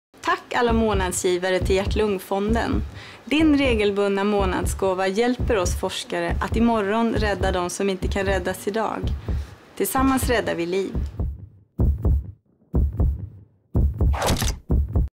Tack alla månadsgivare! (Cecilia Österholm Corbascio)
Cecilia Österholm Corbascio är kärlforskare.